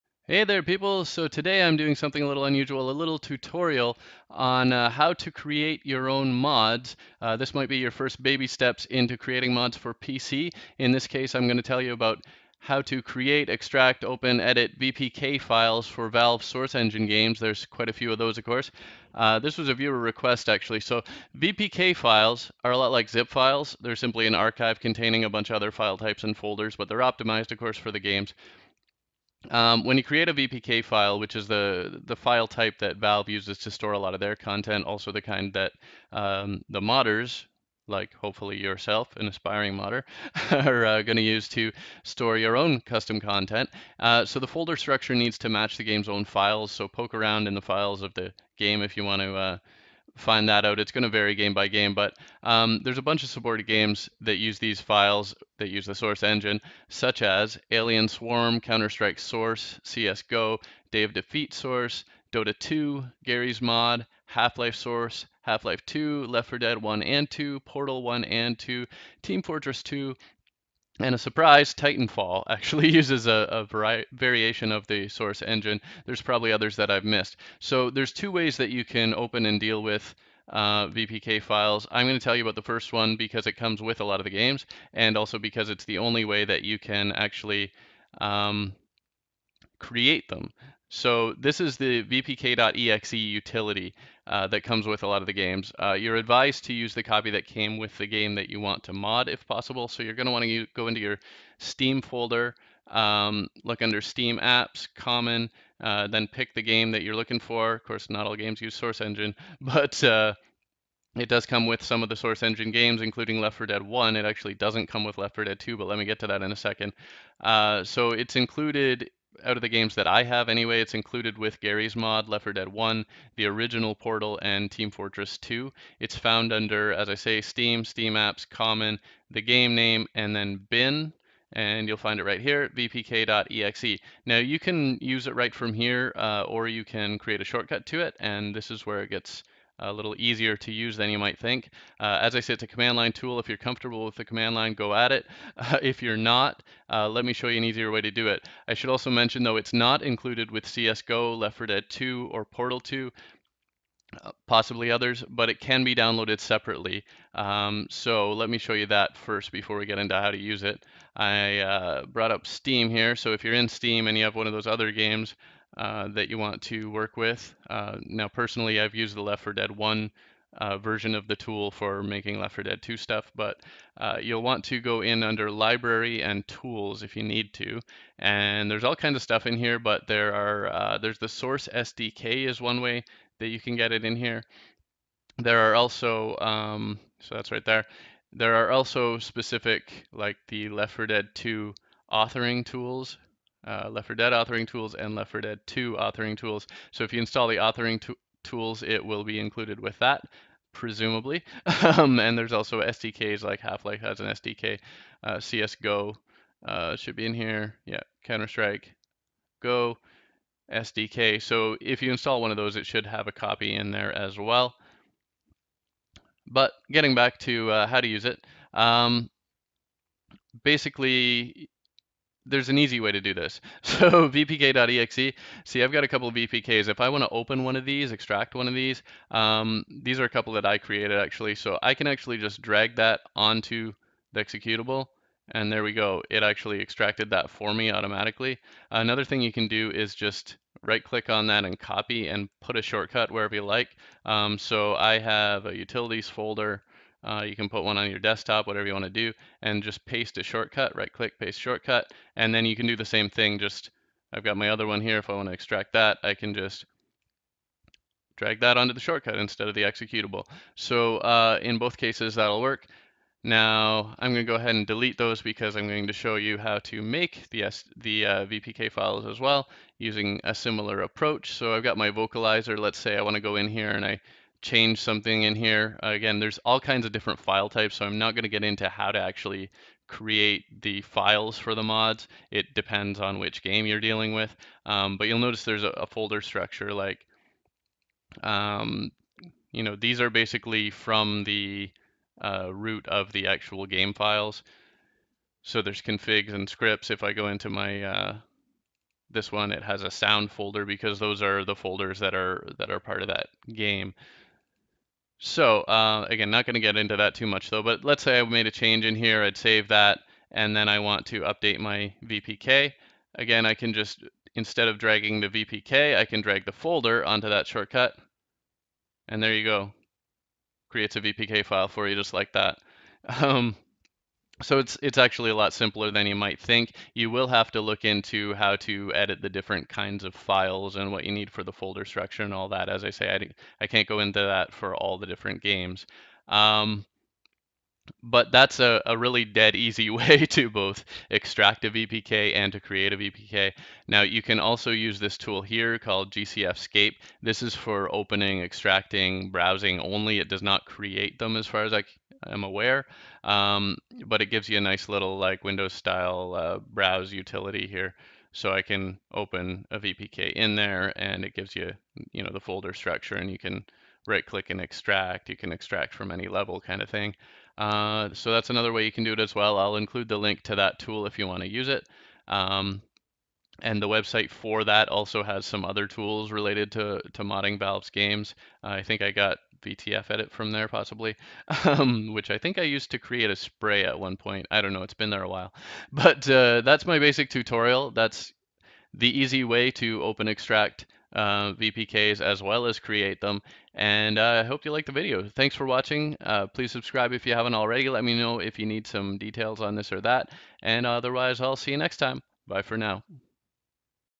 VPK File Creator/Extractor/Viewer & How to make mods for Source Engine games (CSGO, Dota 2, L4D2 PC)
There's video after a bit! Tutorial on how to make a mod for games based on Valve's Source Engine!

This works with many games including CSGO, Dota 2, L4D2, TF2, Garry's Mod, Titanfall and lots of others! I've used it myself to make my own custom mods for the Left 4 Dead games.